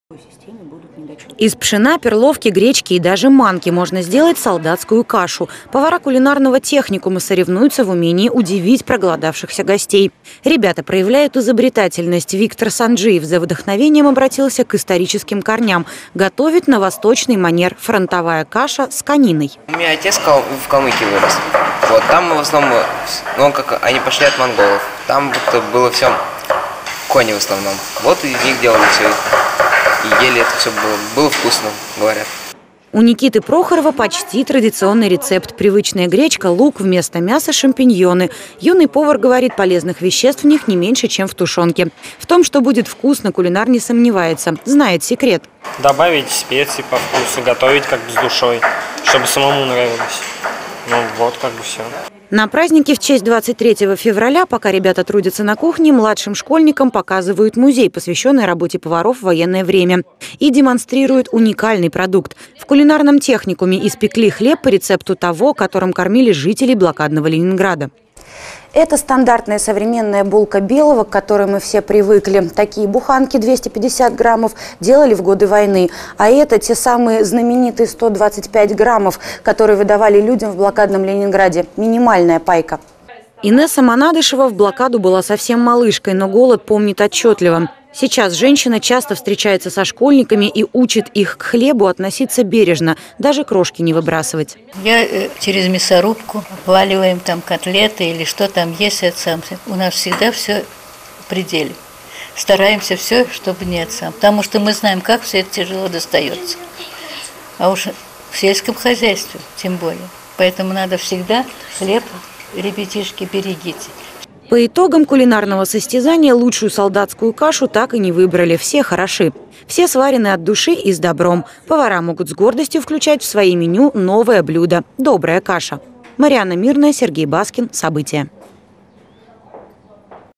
Конкурс рецептов "Солдатская каша"
Ко дню защитника студенты - повара самарского кулинарного  техникума решили приготовить подарок - изобрести новые рецепты солдатской каши.Ну,  и куда же без классики жанра - стандартную гречку с тушенкой тоже не обошли вниманием